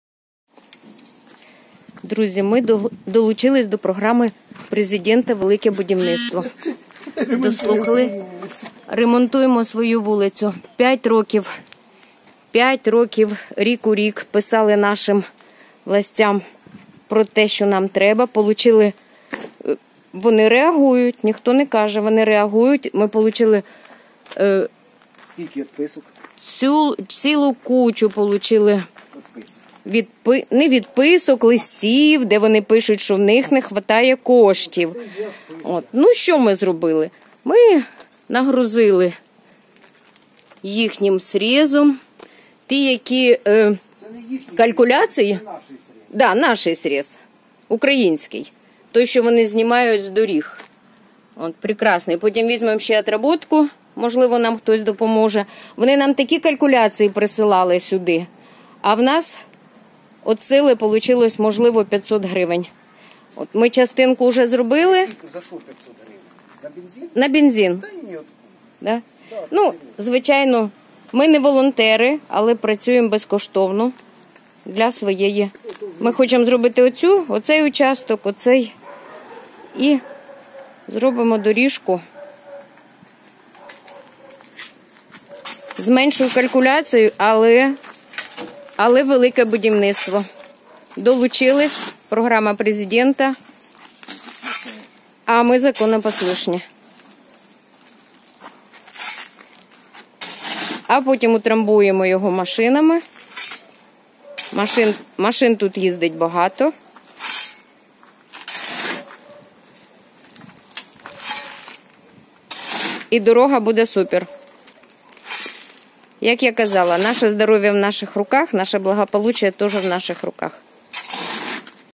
Велике будiвництро
Велике будiвництво. Ми получились до программ президента.